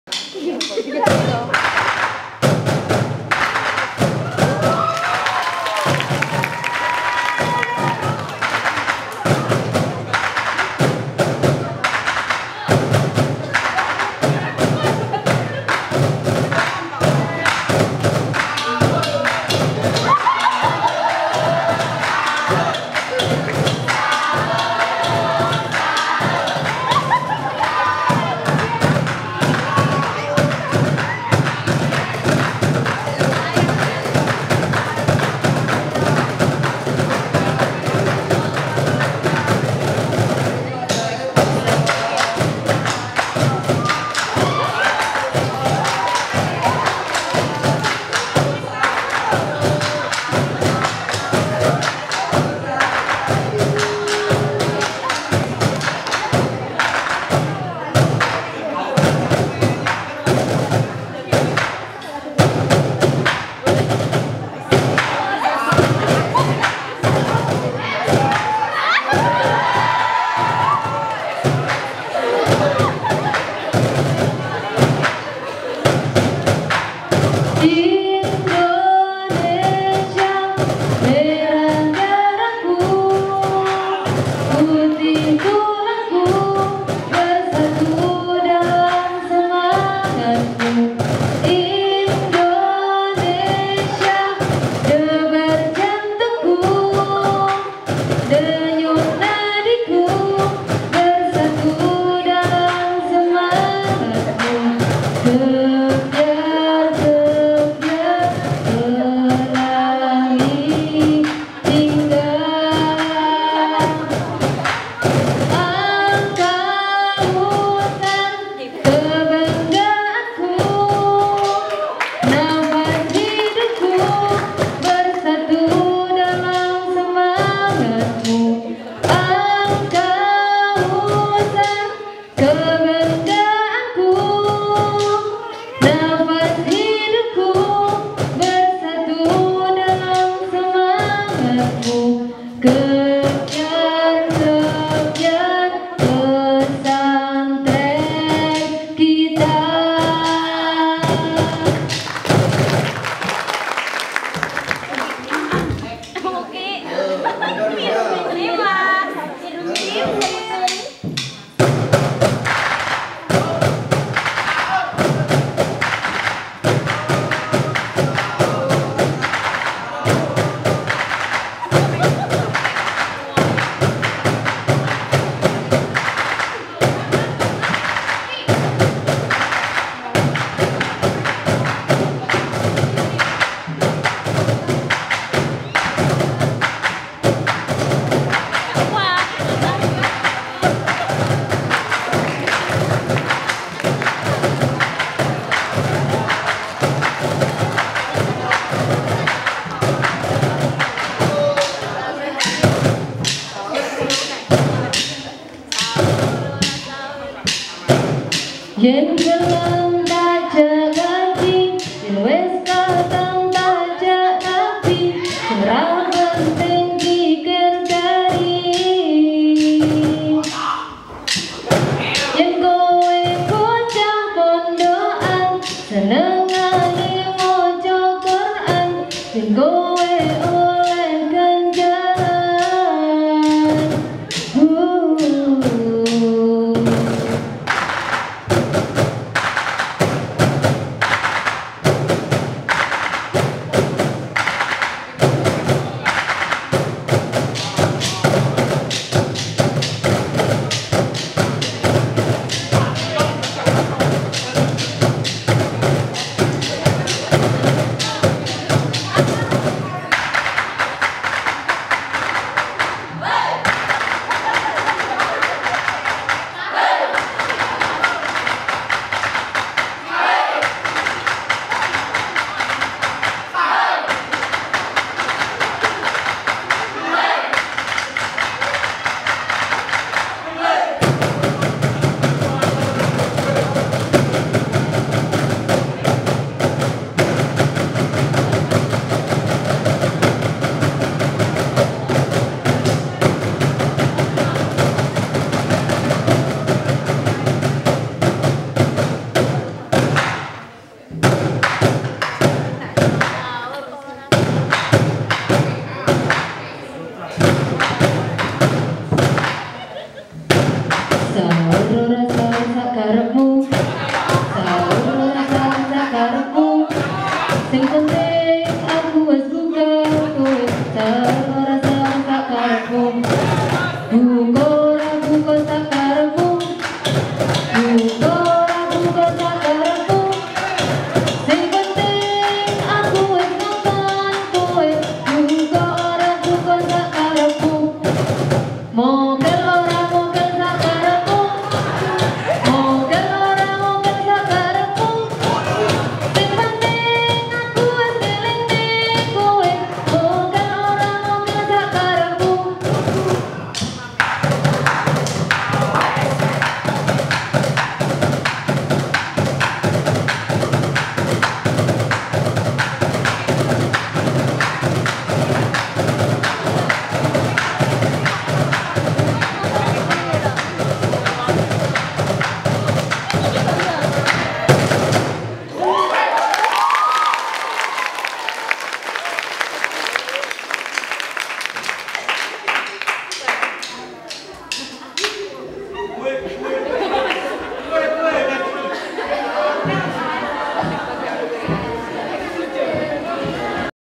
BLACKPINK SANTRI JADI JADIAN
Lomba antar santriwati

Untuk melepas penat selama kegiatan belajar mengajar di pondok pesantren biasnya diadakan lomba kreasi seni dan yang lain sebagainya saat liburan, tentunya agar para santri terhibur, dan inilah salah satu penapilan seru nan lucu kreasi dari gank santriwati BLACKPINK. Santri memang dituntut untuk lues bisa dan biasa apa saja serta tidak boleh lupa bahagia😂

Terimaksih telah menonton😘

    Alula Eva Anjani